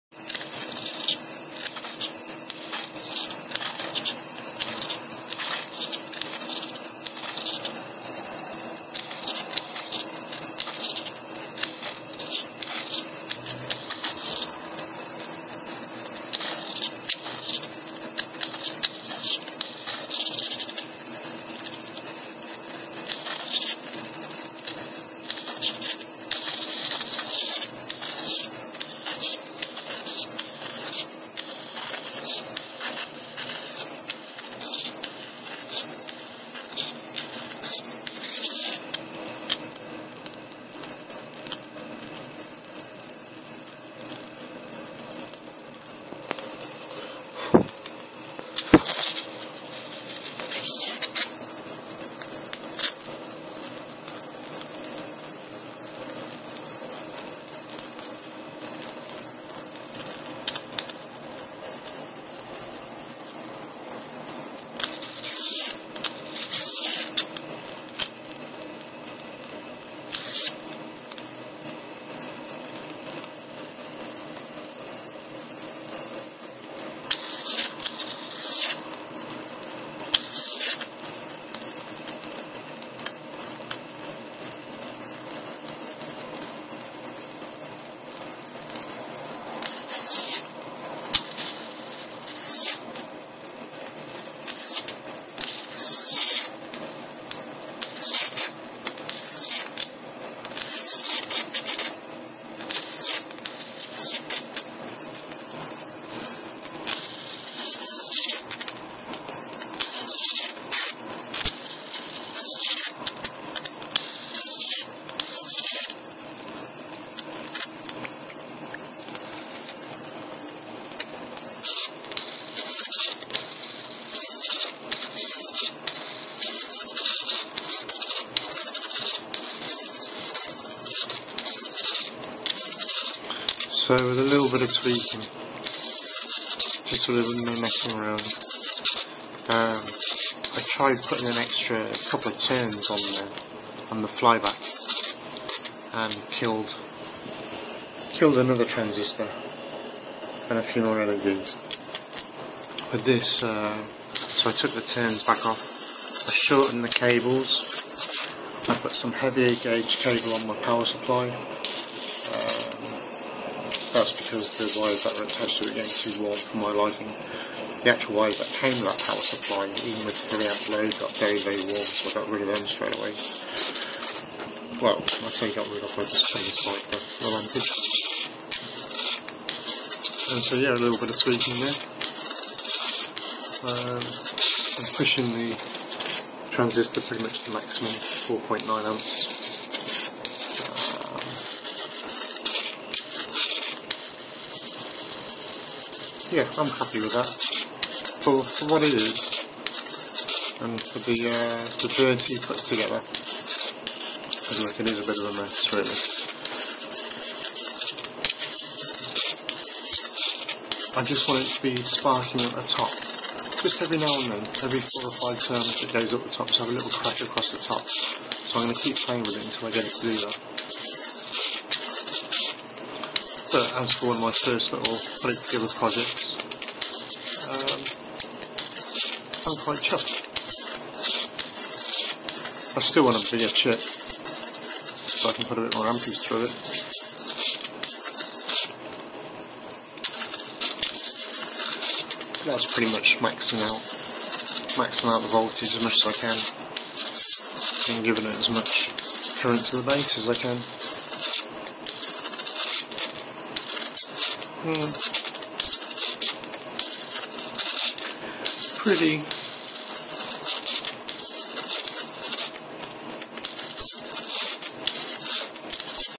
Jacobs Ladder 3
Same flyback circuit but there is an error in it can any spot what is wrong.
I could watch this for hours.